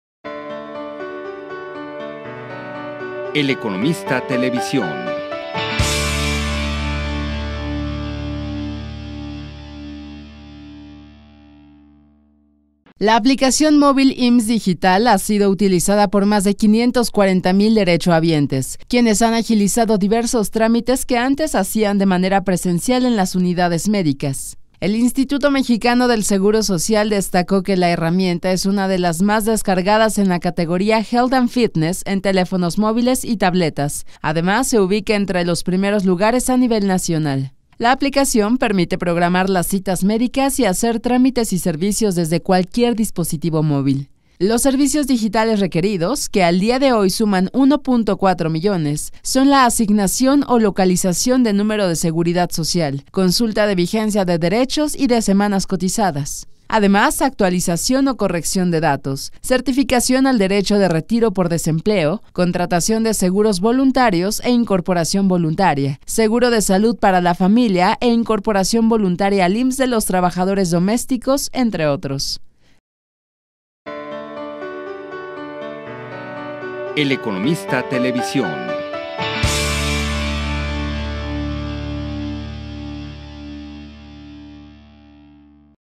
IMSS Digital: trámites desde tu móvil
IMSS Digital es una de las apps más más descargadas en la categoría Health and Fitness en teléfonos móviles tabletas, además se ubica entre los primeros lugares a nivel nacional.